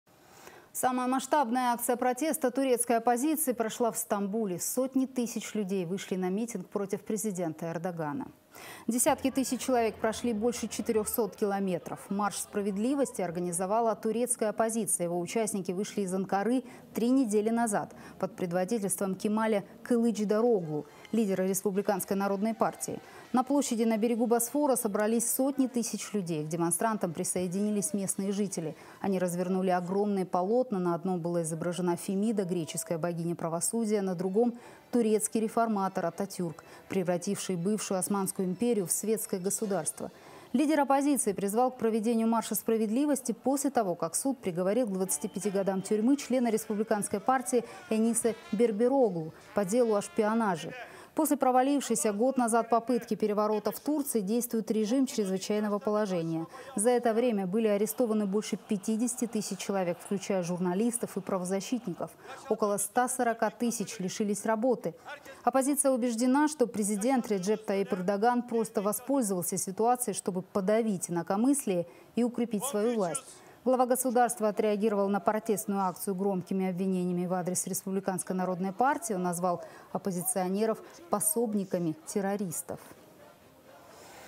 Сотни тысяч жителей Турции вышли на митинг против Эрдогана
Самая масштабная акция протеста турецкой оппозиции прошла в Стамбуле. Сотни тысяч людей вышли на митинг против президента Эрдогана.